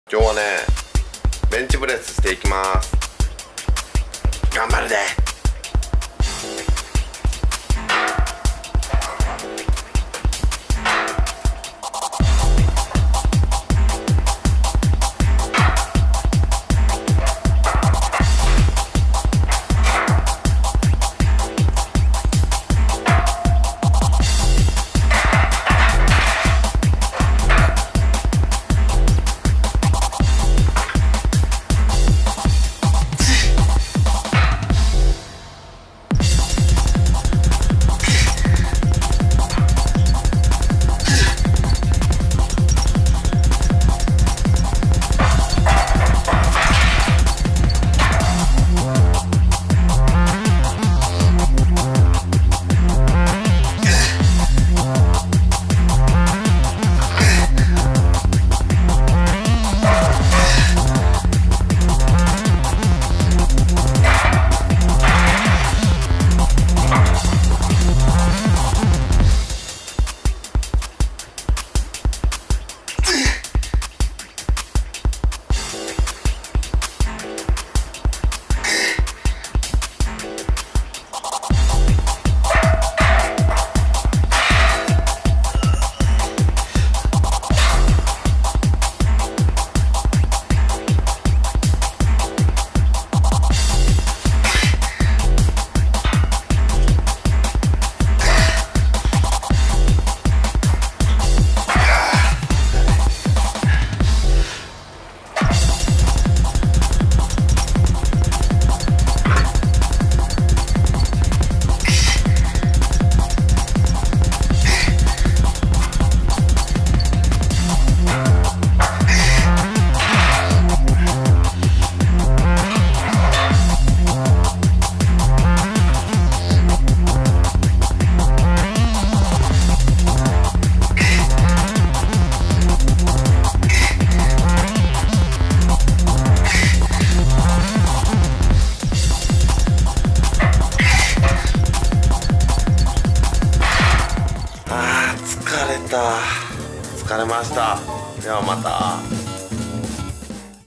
[筋トレ] ベンチプレス頑張っていい体になるのだ｜Muscle training Bench press
ベンチプレスしました
トレーニング内容は
40×10　70×10　100×1　125×1
110×6　110×5　110×5　110×3
105×4　100×6
です
もっともっと上げれるようになりたいよー
頑張ります
動画内楽曲

使用フリー音楽素材・効果音素材　TAM Music Factory